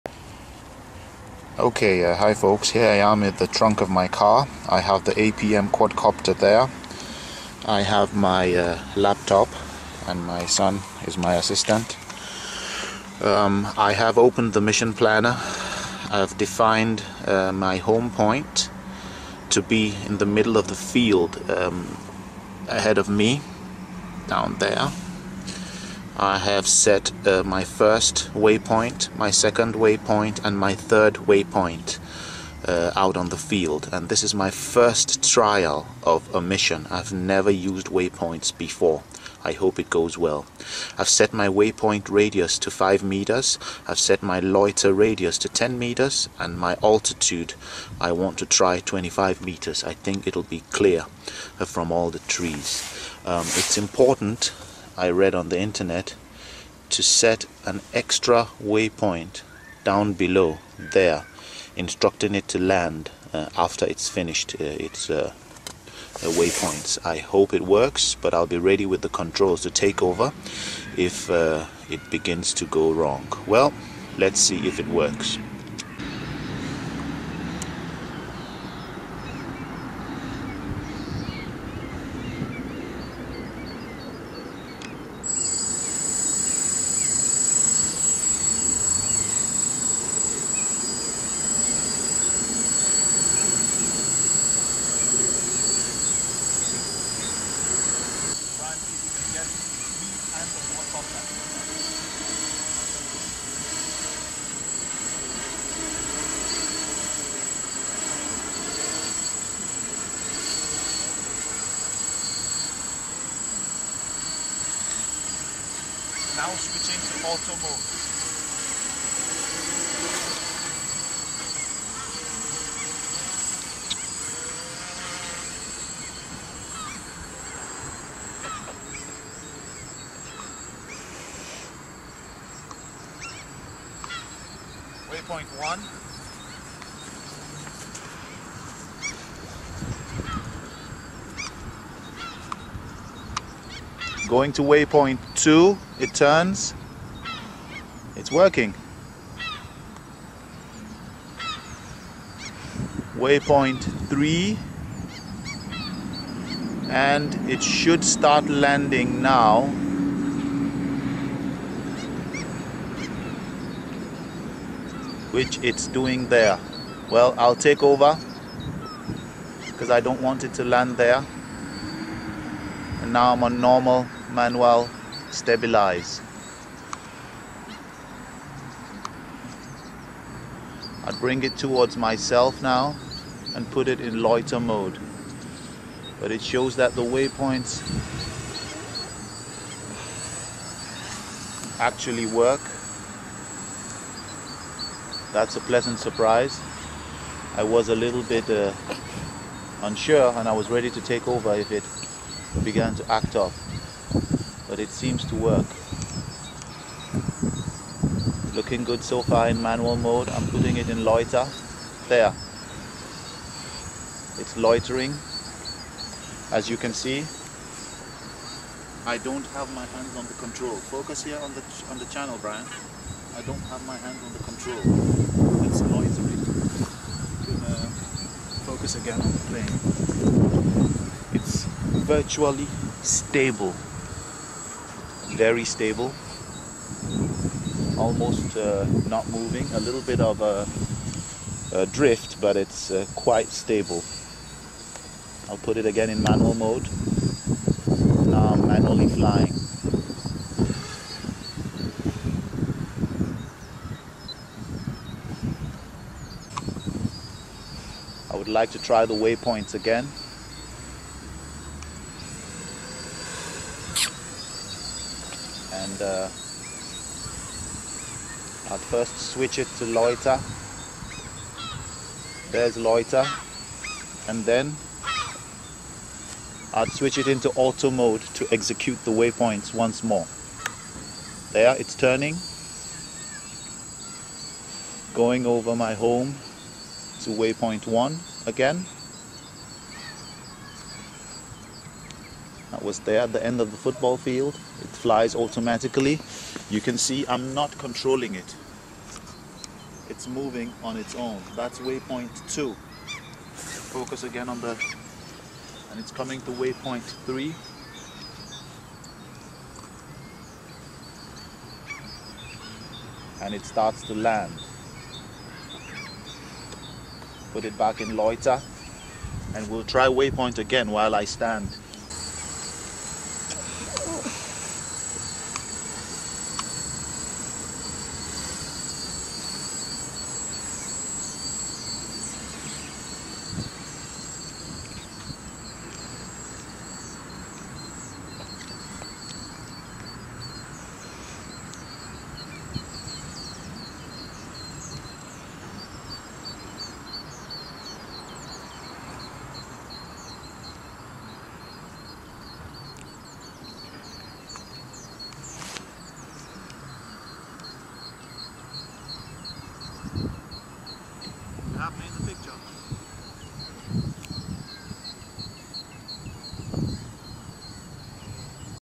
Unmanned drone Waypoint Mission planner test
My first test of the mission planner waypoint function of the Arducopter flight controller software. Please excuse the lack of focus in some parts of the video, the video is being taken by my 8 year old son who is still learning to operate the video camera properly.
Frame: F450
props: APC 10x45
Motors: el cheapo EMC chinese 750 kv
ESC: DJI OPTO 30A
Flightcontroller APM 2.5 running firmware Arducoter 2.1.9b